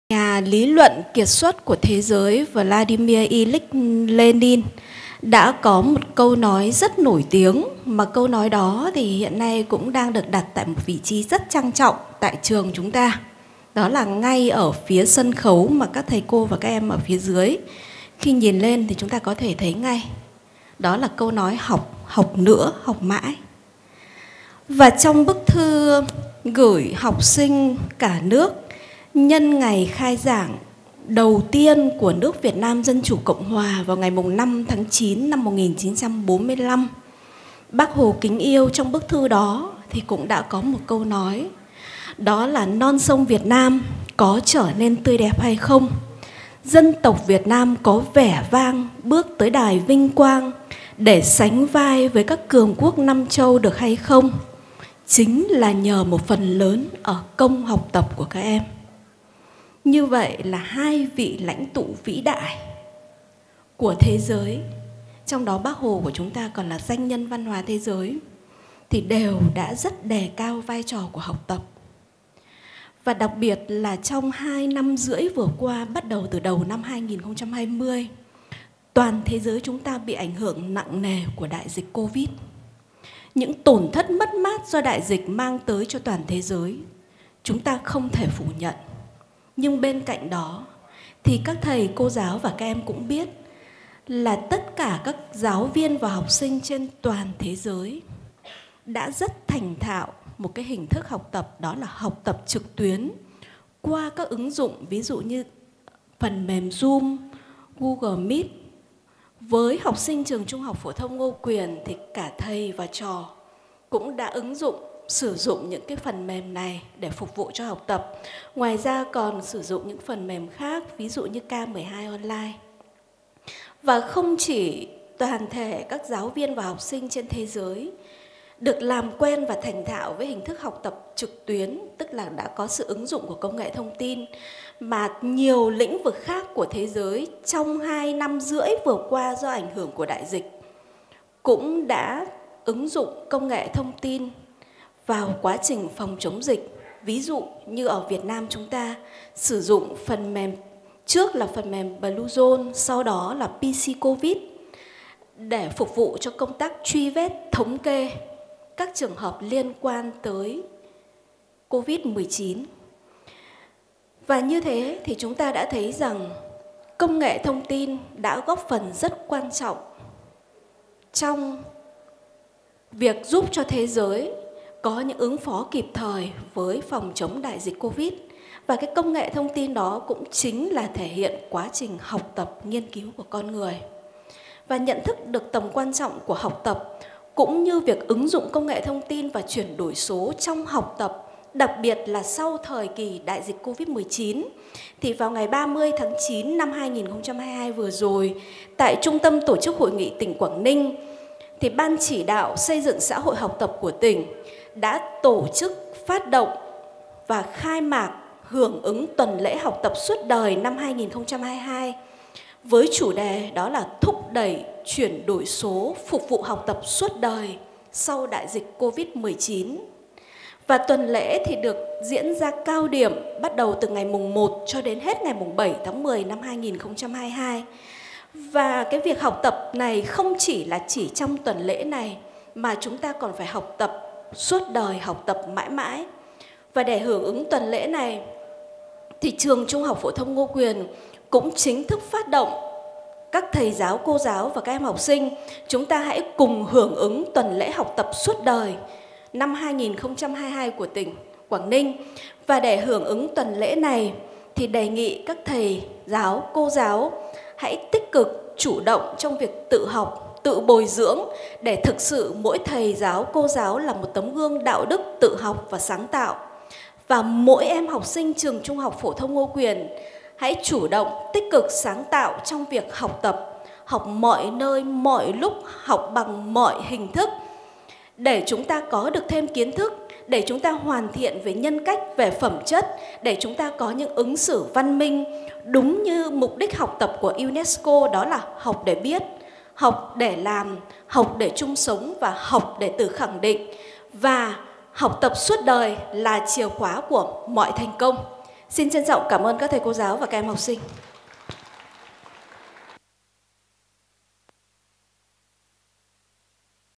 Phát động “Tuần lễ hưởng ứng học tập suốt đời”, THPT Ngô Quyền, Quảng Ninh
Ngày 3/10/2022, Trường THPT Ngô Quyền tổ chức  phát động “Tuần lễ hưởng ứng học tập suốt đời” năm 2022 với chủ đề "Thúc đẩy chuyển đổi số phục vụ cho học tập suốt đời sau đại dịch COVID-19"
Tham dự: Ban Giám hiệu, toàn thể Cán bộ, giáo viên, nhân viên và Học sinh trường THPT Ngô Quyền
Phát biểu Phát động: Cô Mai Thùy Dương, phó Hiệu trưởng
Camera: CLB truyền thông
Âm thanh: Hoàng Anh, Đình Quân